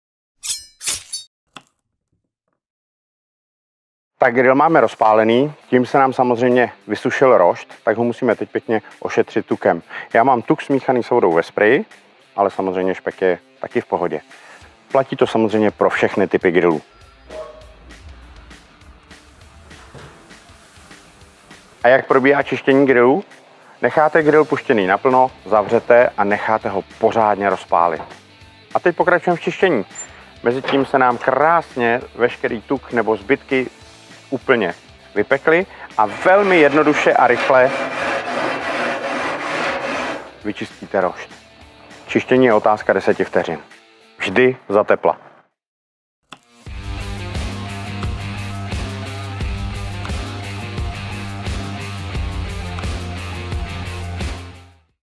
Jak čistit gril
Grilování je voňavý zážitek, který může dost dobře pokazit neudržovaný gril. Zde nejde jen o nevábně působící připálené zbytky masa z předešlého grilování, ale o vaše zdraví! Pár jednoduchých, ale velmi důležitých rad najdete ve videu. Z chutí do toho a půl je hotovo!

Navštivte naši webovou stránku kde budeme přidávat spoustu užitečného a zajímavého obsahu.